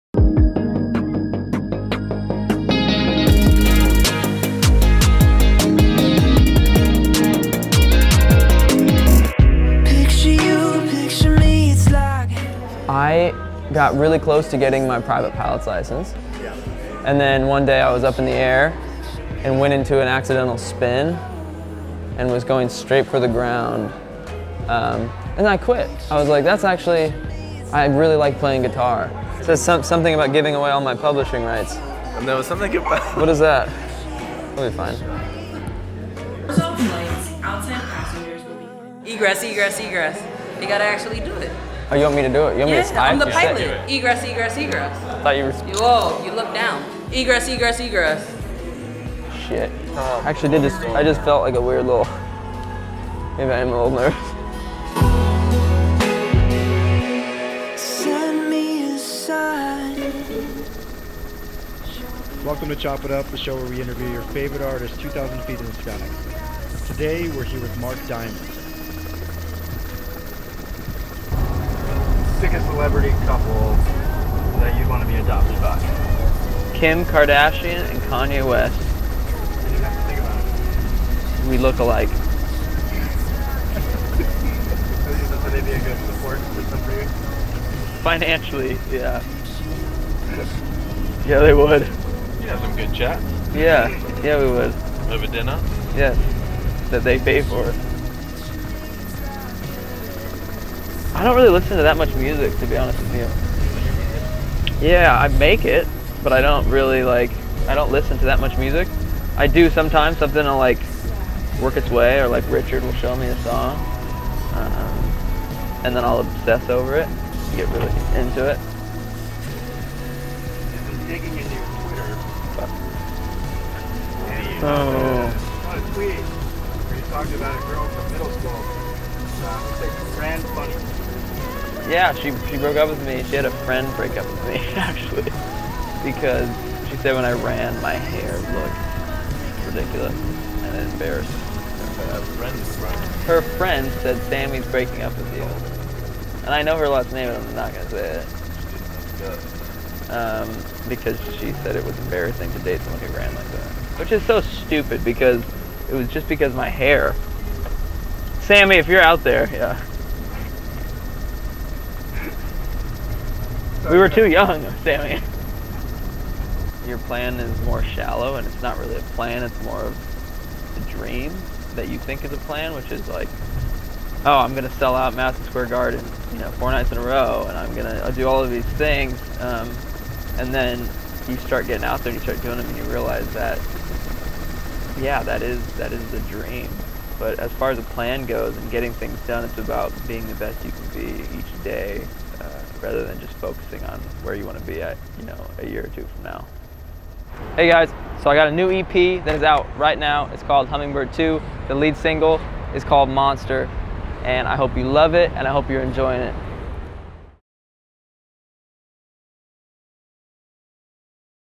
Mark Diamond dropped out of flight school but then still goes up in a helicopter (INTERVIEW)
Shot by Mike Klubeck, Nick Freeman, Giles Perkins
Edited by Giles Perkins
Produced by °1824
Special thanks to FlyNyon